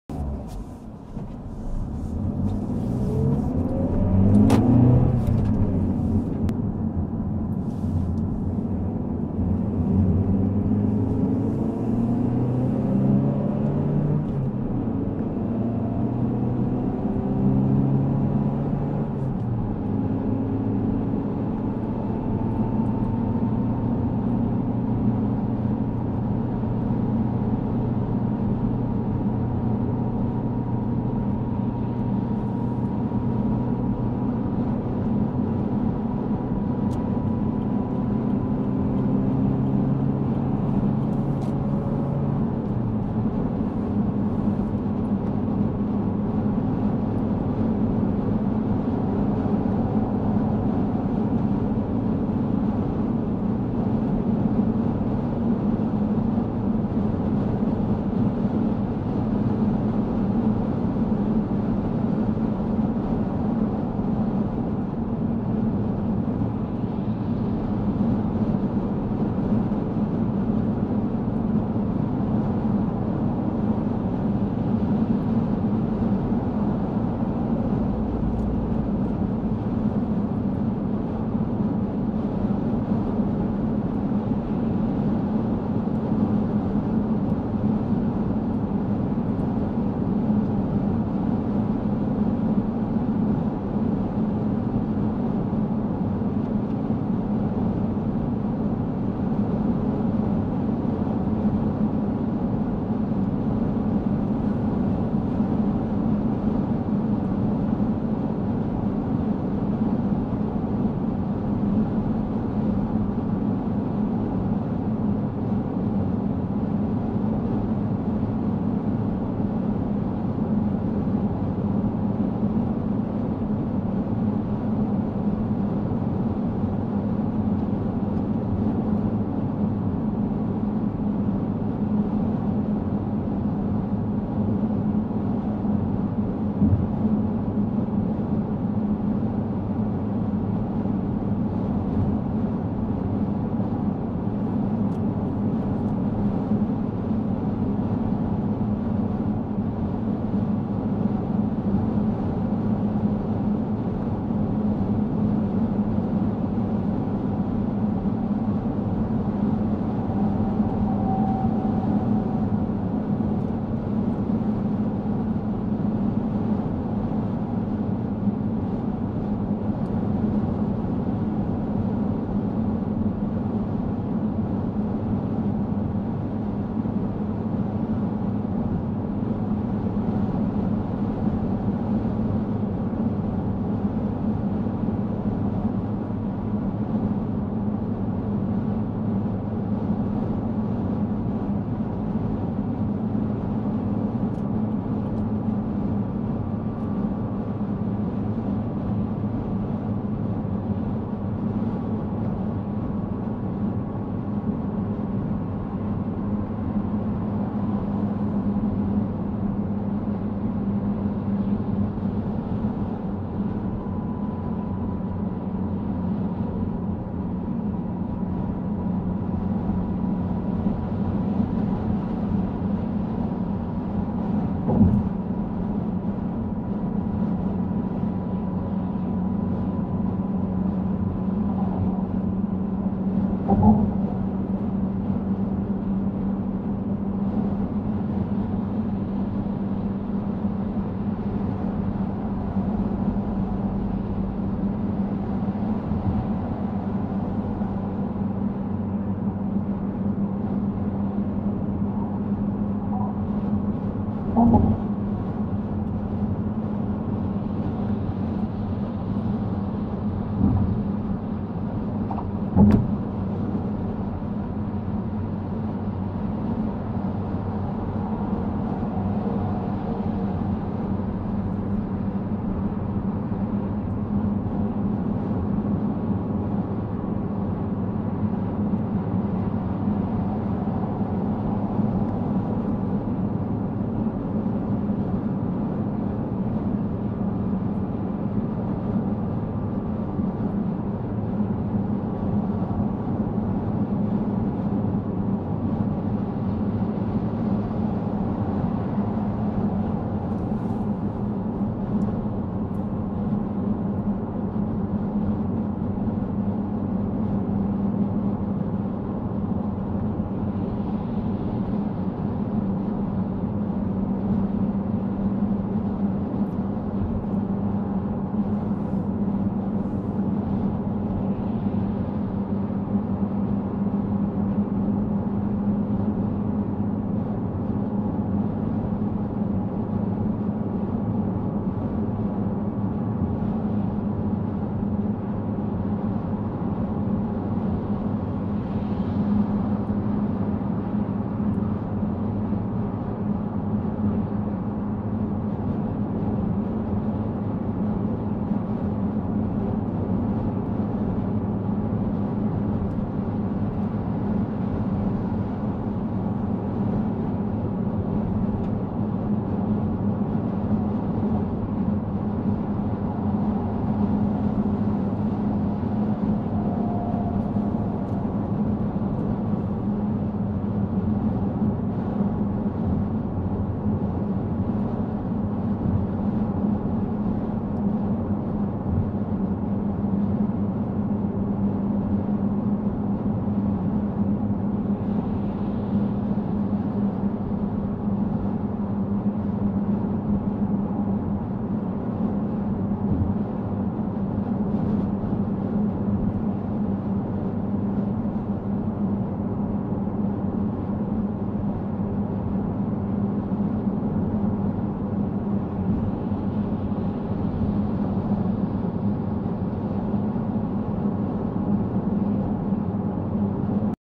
Driving over the Little Belt Bridge ( Lillebæltsbroen ) 2018
Lillebaelt Bridge, Denmark, from Lillebælt-cafeteria to Fredericia.
Lillebæltsbroen fra Lillebælt-cafeteriet til afkørsel Fredericia

Driving across the Little Belt Bridge from Funen to Jutland
The New Lillebælt ( Little Belt ) Bridge from 1970 crosses the Little Belt strait between Jutland (Jylland) and the island of Funen (Fyn) in Denmark. The bridge is 1700 metres long, the main span is 600 metres, the pylons is 120 metres, and the maximum clearance is 44 metres.

6. april. 2018
Strækning fra Lillebælt-cafeteriet (Fyn) til afkørsel Fredericia V
Den nye Lillebæltsbro er en sekssporet motorvejsbro mellem Jylland og Fyn indviet 21. oktober 1970.
Broen er 1.700 meter lang, 33,3 meter bred og har et frit spænd på ca. 600 meter. Gennemsejlingshøjden er 44 meter.

Recorded on Microsoft Lumia 950